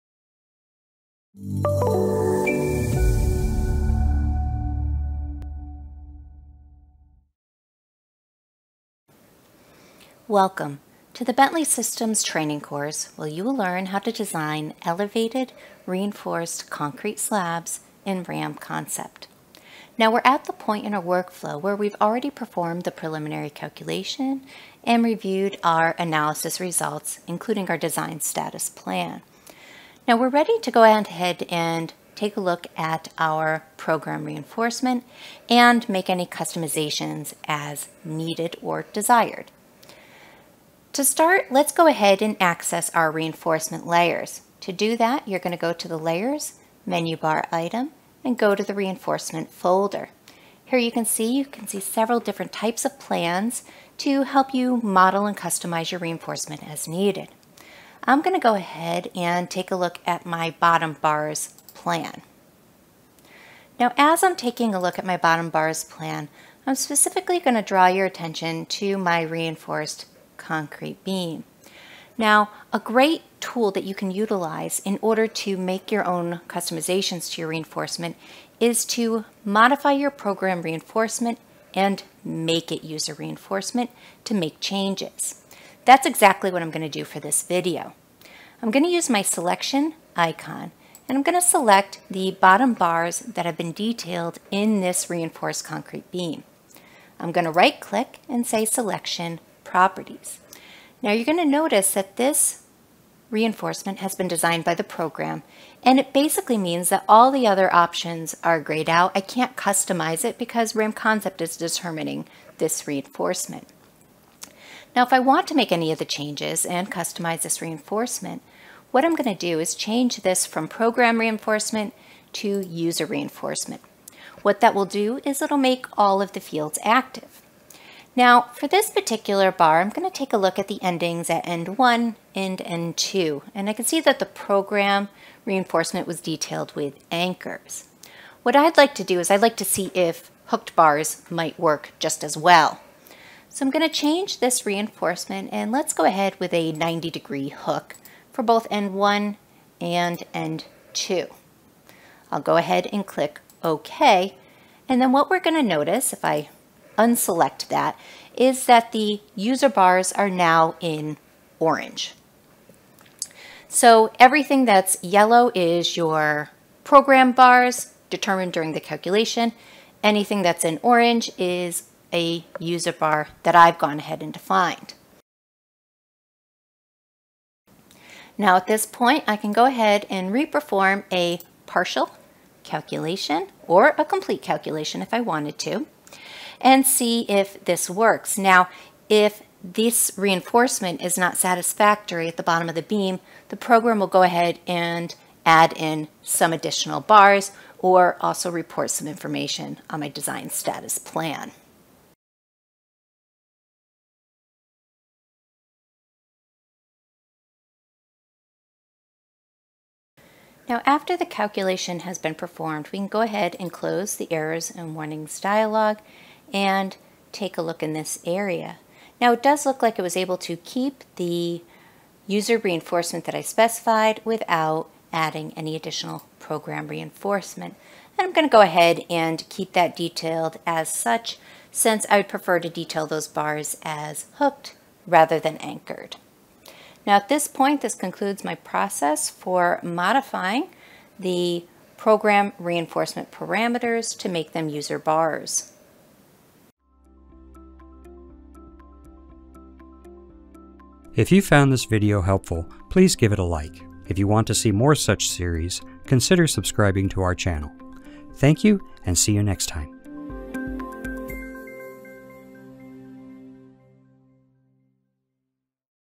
Modifying the Program Reinforcement in RAM Concept
In this video, you will learn how to modify the program reinforcement in RAM Concept.